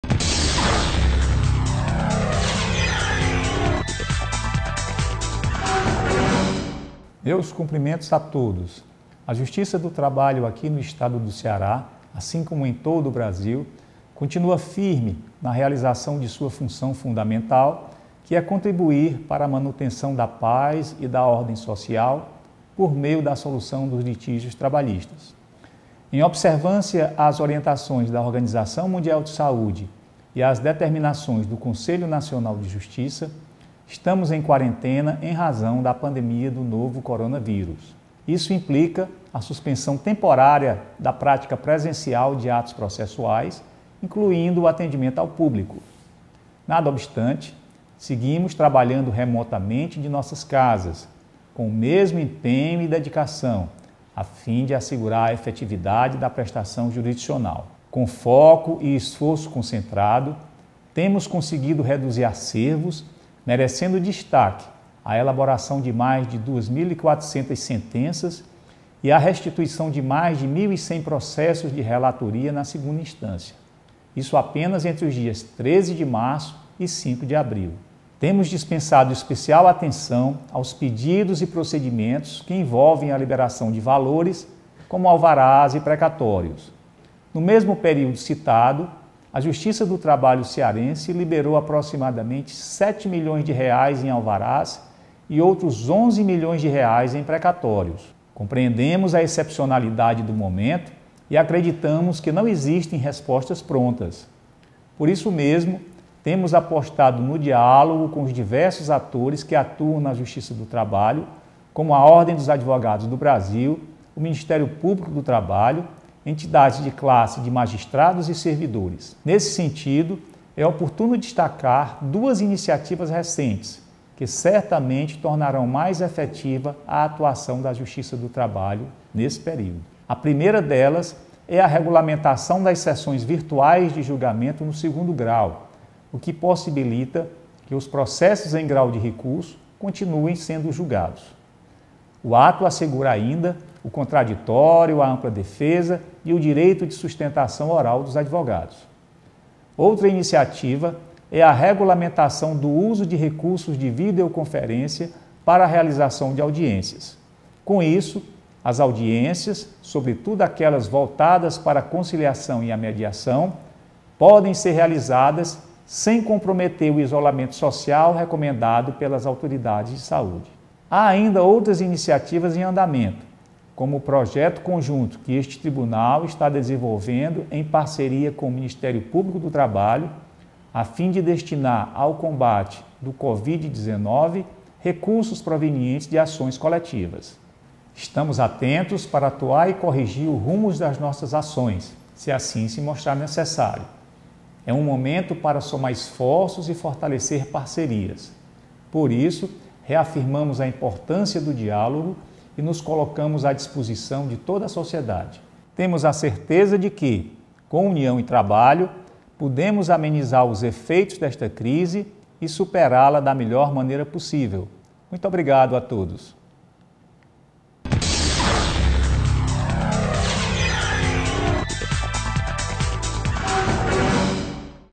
Presidente do TRT7 e o funcionamento da Justiça do Trabalho do Ceará durante crise do coronavírus
Pronunciamento do desembargador Plauto Porto, presidente do Tribunal Regional do Trabalho da 7ª Região (TRT/CE) sobre as medidas tomadas durante a crise da covid-19.
Para combater o contágio do coronavírus, desde o dia 13 de março de 2020 os magistrados, servidores e estagiários da Justiça do Trabalho do Ceará passaram a realizar suas atividades de forma remota, em regime de teletrabalho. Com a suspensão do atendimento presencial, o TRT/CE adotou a possibilidade de realização de audiências por videoconferência e sessões virtuais de julgamento.
Os dados estatísticos com a produtividade do TRT/CE durante a pandemia do coronavírus está disponível em um painel no no menu Covid-19: Produtividade.